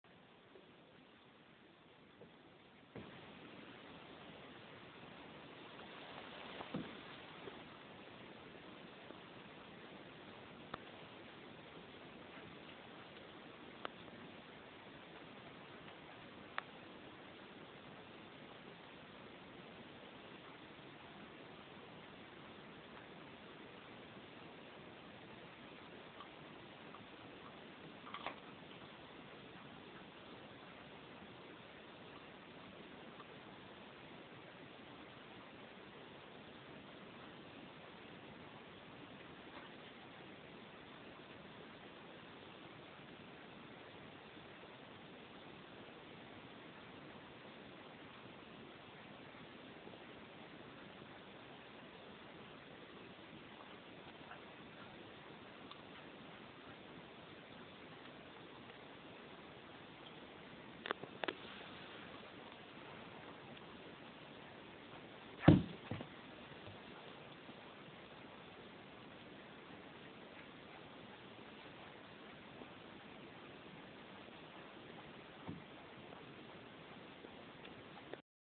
Canine Epileptoid Cramping Syndrome (CECS)
Murphy my border terrier, example of a CECS episode.